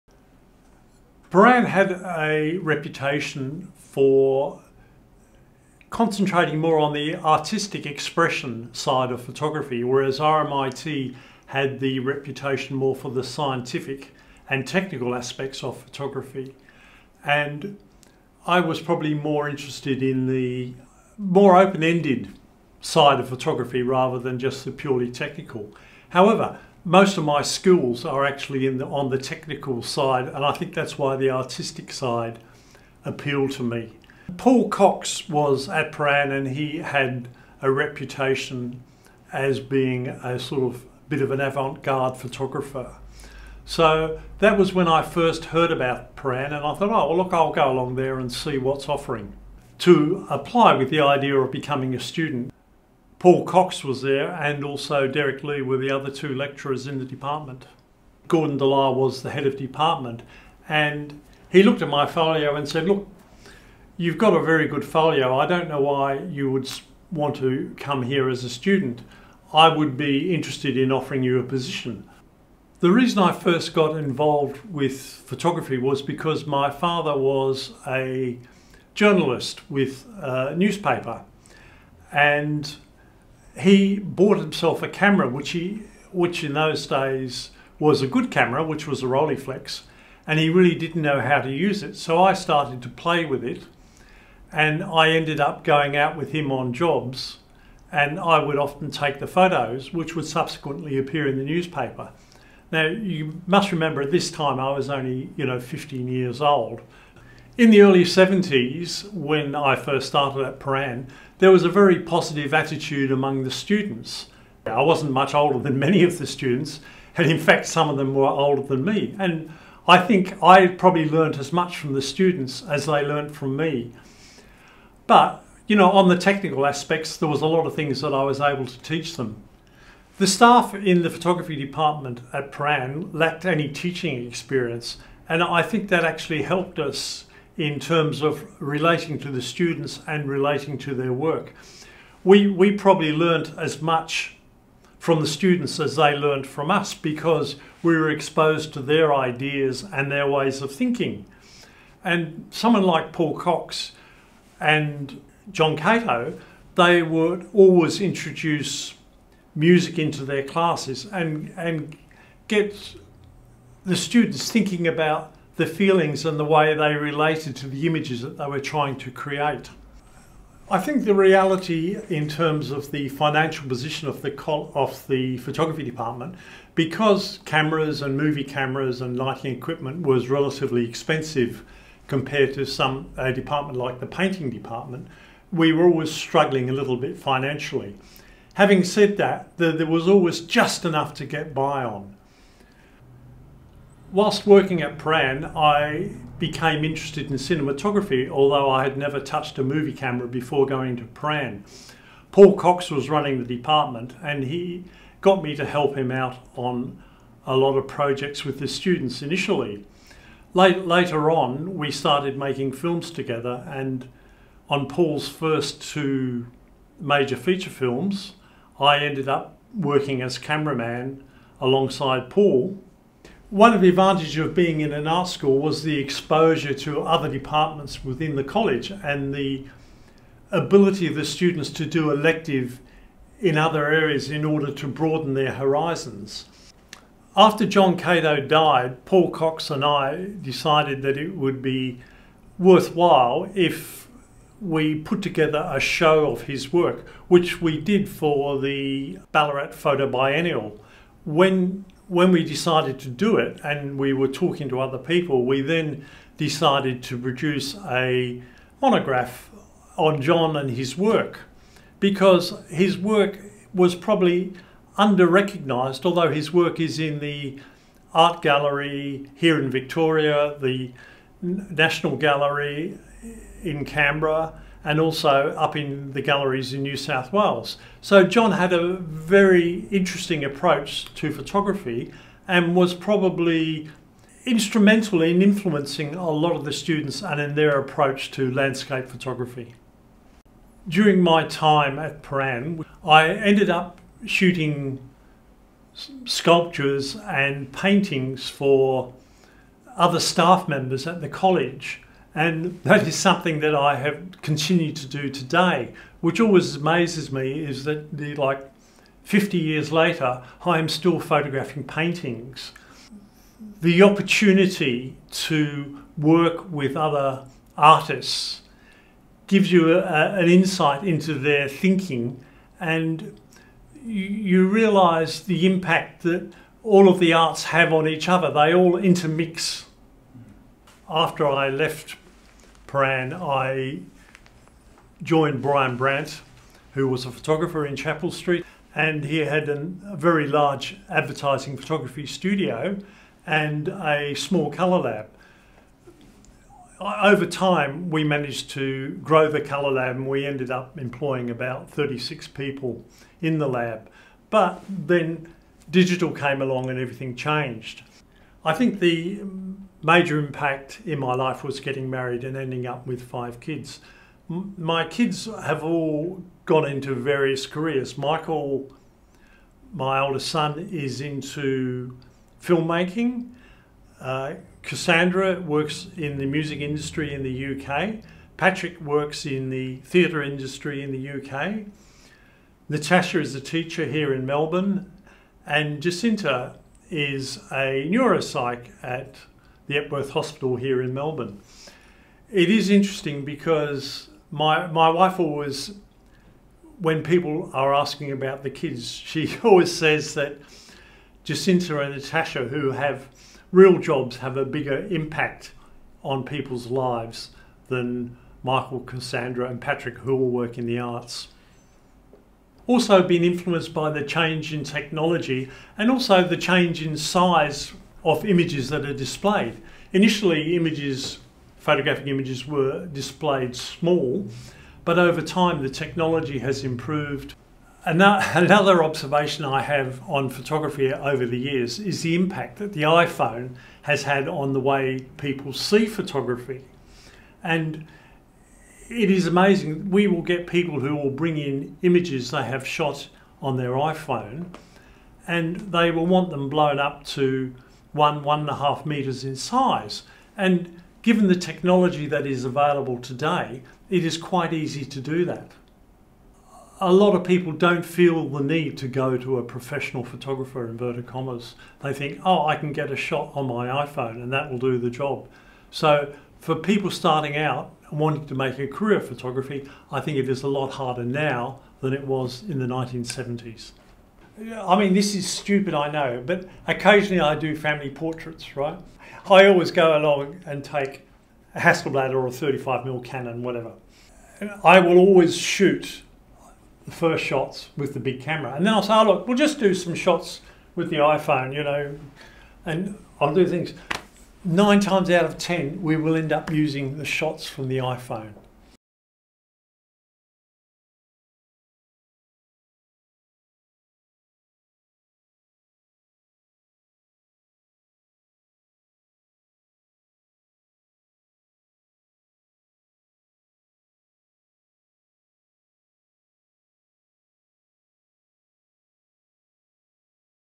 Bryan Gracey, Lecturer Photography 1970 to 1980 Prahran CAE
Interview taken on Wednesday 13th of March 2024
Duration 8:40 minutes
Production: Peter Leiss & assisted by Nicholas Nedelkopoulos
Image Contributors:
Andrew Chapman
Rennie Ellis
Bryan Gracey
Peter Leiss
James McArdle
Jim McFarlane
Rod McNicol
Greg Neville
John Cato
Images from, Proteus, Seawind, Tree – a journey #13 +, Petroglyph #12 +